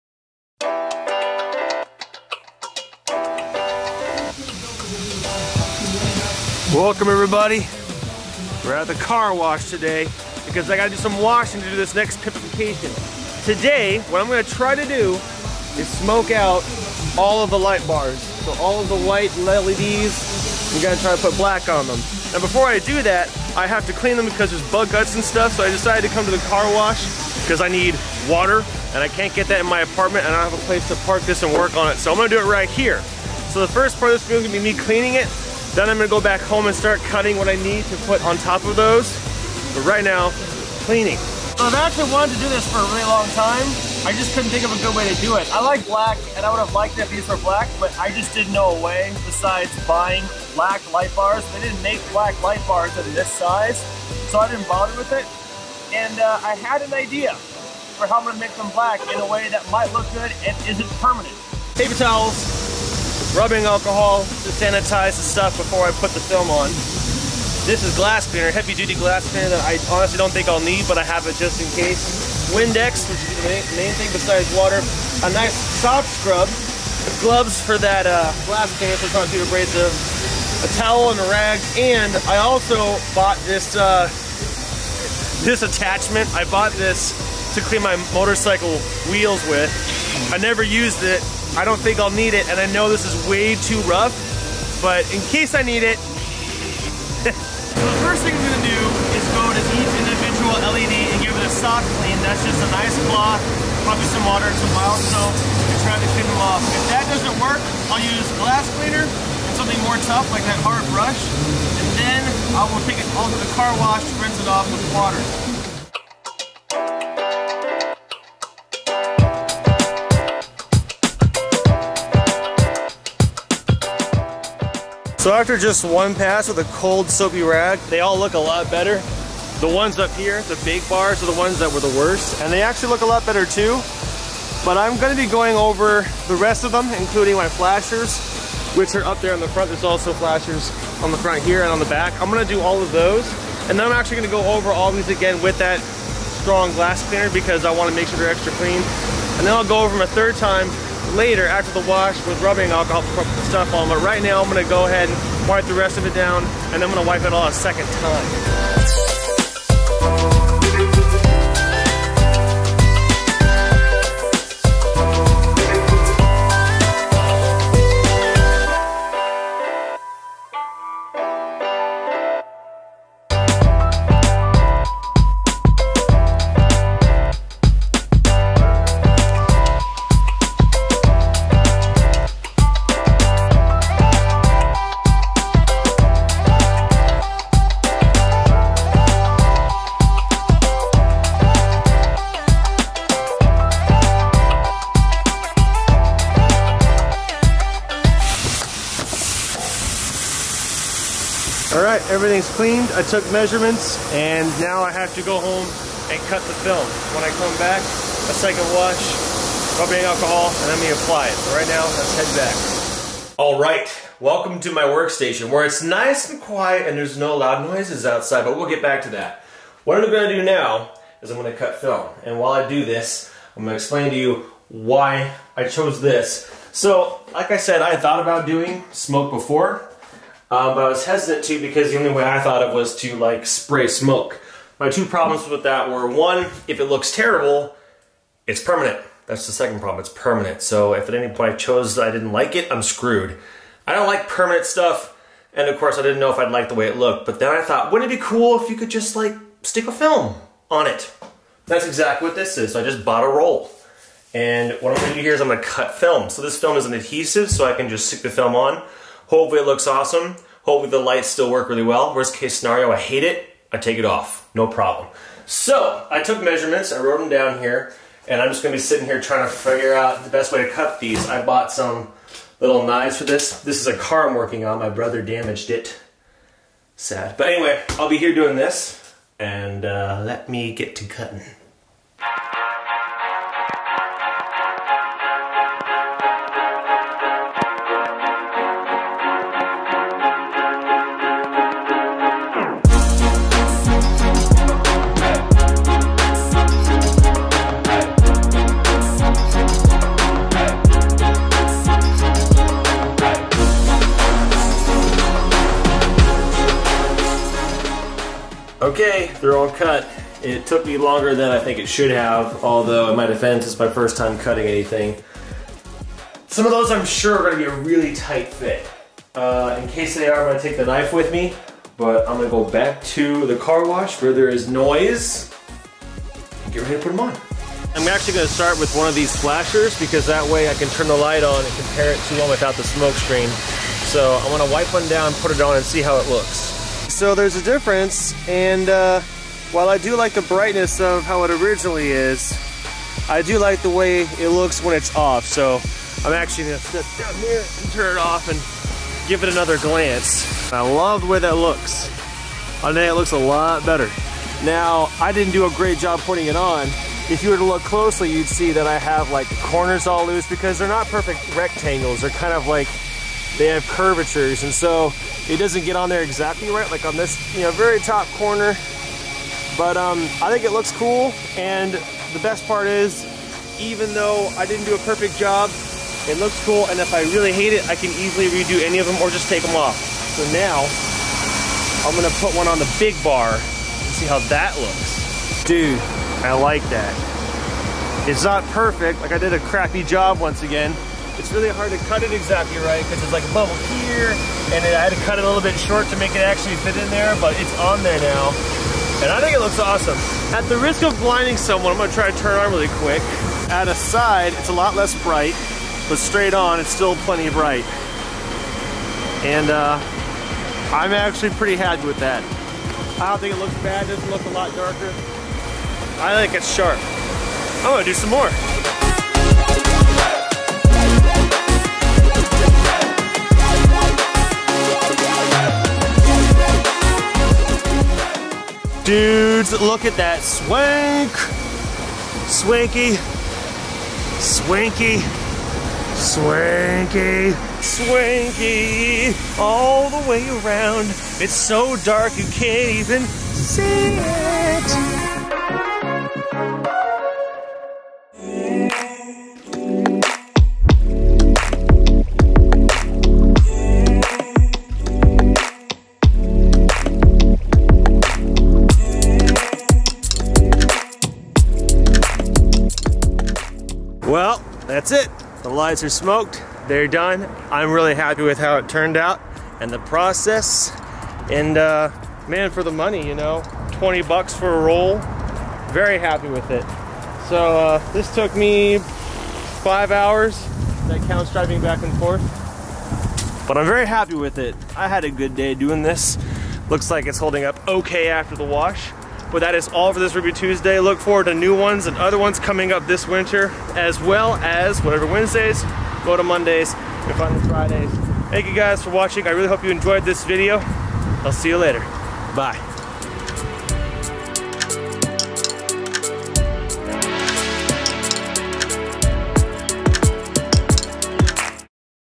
Rubi Tuesday #12 - Light Bar Tinting
This is something I've wanted to do for a long time! I always thought about spraying the light bars with smoke paint, but I chose not to because it's permanent and I didn't know if I'd like it.

Well, one day, I got the bright idea to just use adhesive tint film and I think it worked out great! I could've done a better job, but it was my first time doing something like this and I'm really happy with it. Seriously, the roll was 13 bucks and I still have plenty left over. Here's the link to what I bought if you want to try something like this on your car or bike